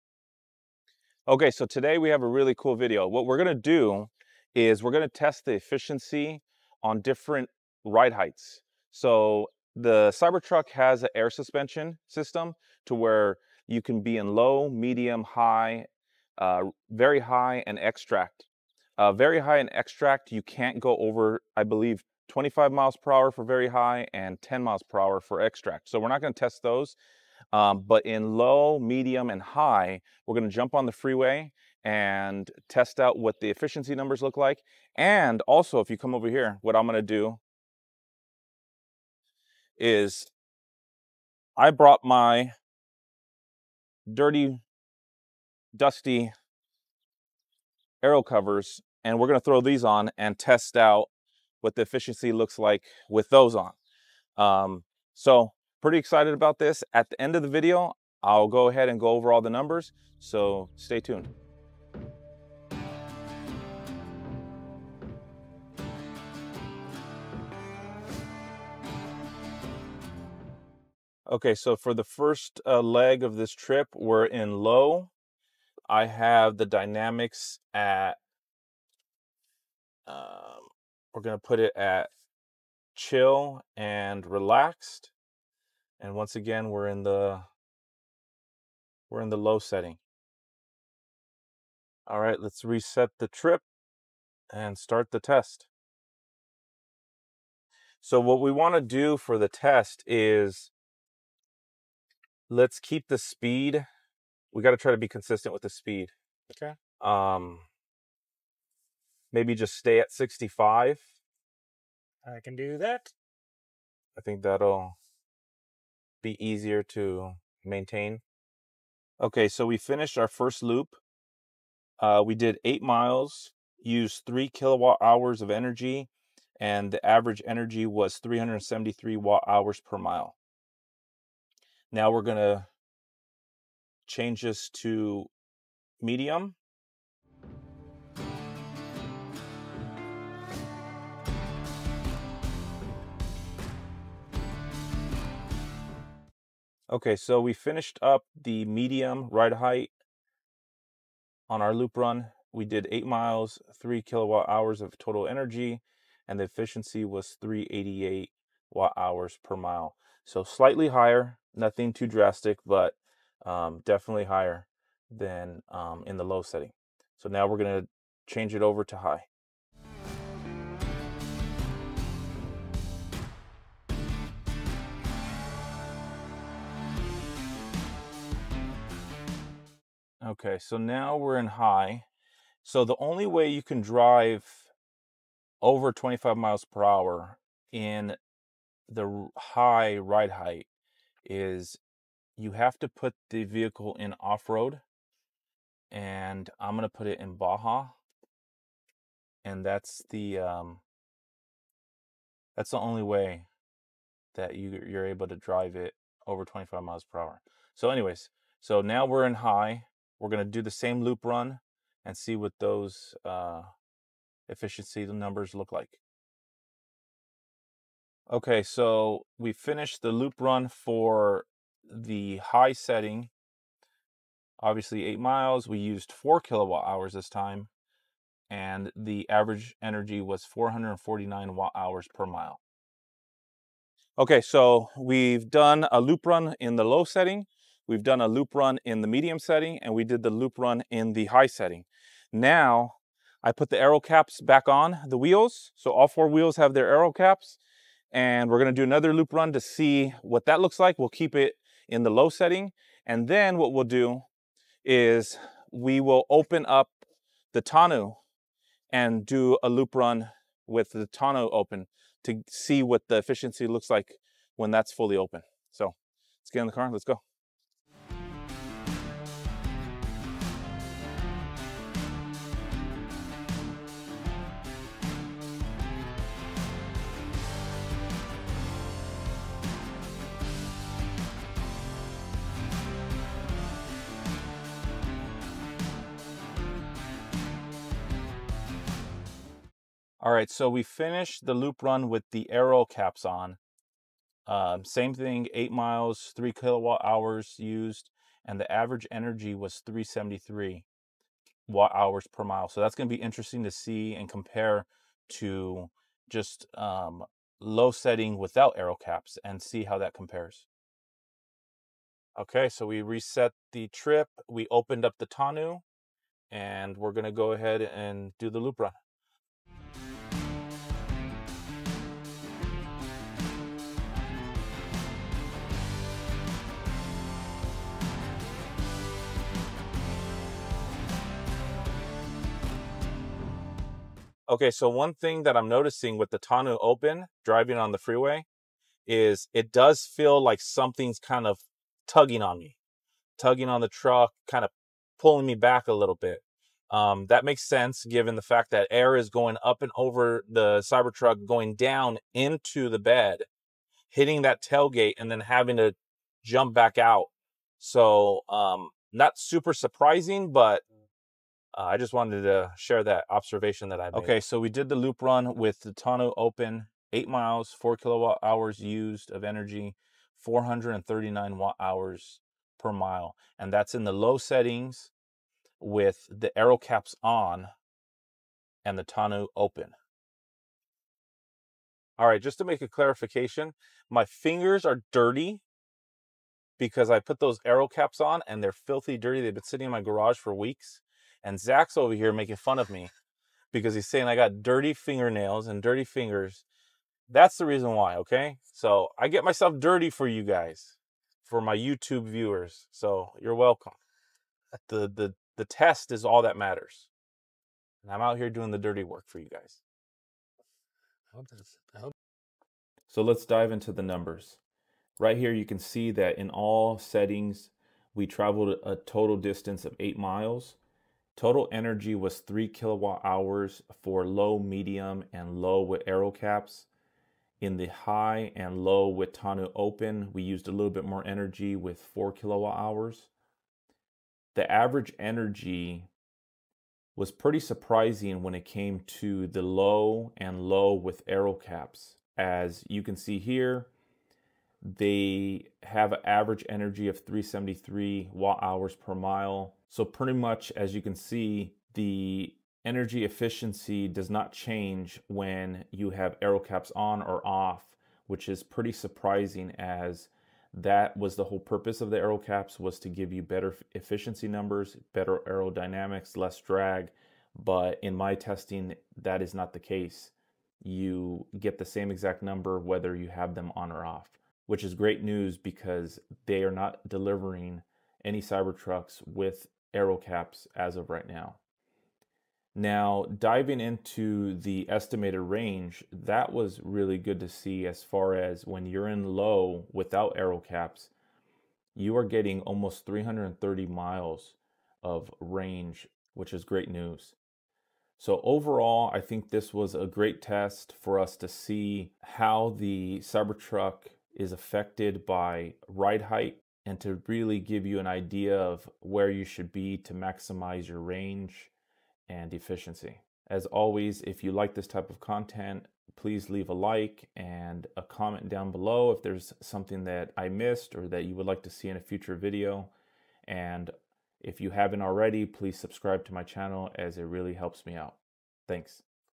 Shocking Results of CyberTruck Suspension Test
Join us in this intriguing exploration into the world of electric vehicle (EV) efficiency and range, as we delve into the significance of the CyberTruck's suspension ride height. In this video, we analyze whether the CyberTruck's suspension settings play a crucial role in determining its energy consumption and overall range.

With the rising popularity of electric vehicles, understanding the factors that influence their efficiency has become paramount. The CyberTruck, Tesla's highly anticipated all-electric pickup truck, boasts innovative features, including adjustable suspension settings. But does altering the ride height affect its efficiency and range? That's what we aim to uncover.

Through detailed analysis and real-world testing, we'll examine the relationship between suspension ride height and energy consumption. We'll discuss how different settings may impact aerodynamics, rolling resistance, and overall performance, shedding light on the intricate balance between comfort, capability, and efficiency.

Whether you're a CyberTruck enthusiast, an EV owner, or simply curious about the future of automotive technology, this video offers valuable insights into the intersection of design, engineering, and sustainability in the realm of electric vehicles.

Don't miss out on this informative journey into the world of CyberTruck suspension and its implications for EV efficiency and range. Hit play now and join the discussion!

ButterEVon50 for $50 off

0:00 Intro
1:02 Suspension in Low
2:17 Suspension in Medium
2:49 Suspension in High
3:53 Suspension in Low with AeroCaps on
5:11 Suspension in Low with Tonneau Open
6:52 Final Numbers